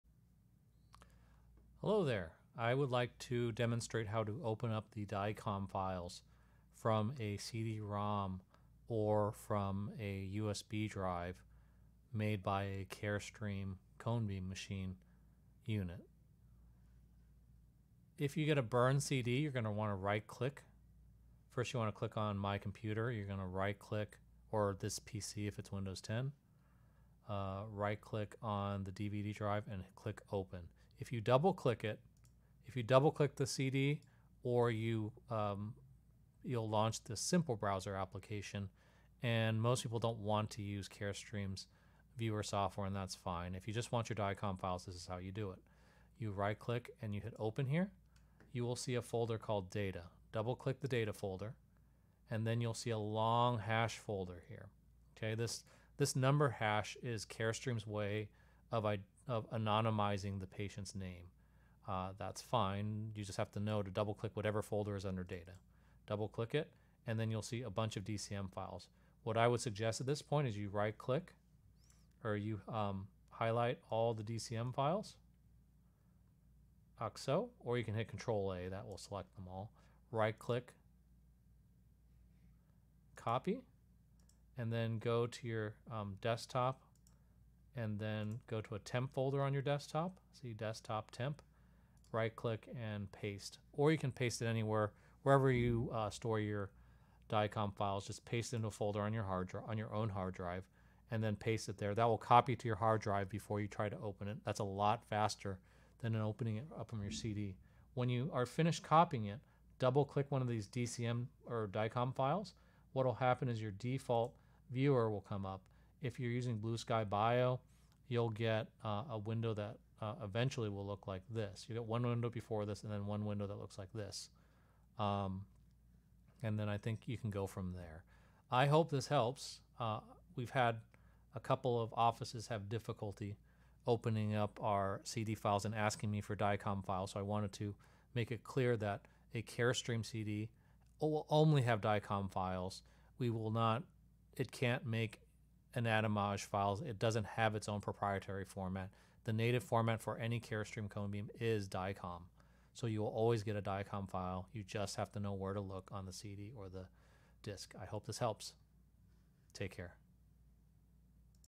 How to find the DICOM files from a Carestream generated CD-R or DVD-R
Insert your CD or DVD
Open up "This PC" or "My Computer on your windows PC.
Right click the DVD drive
Select "Open" from the right click menu
Double click on Data
Click on the long numerical hash folder
Highlight the DCM files
Right click, copy
Go back to your computer and make a new folder
Go into the folder, and right click, paste
Double click the newly pasted dicom files to open up your default implant software.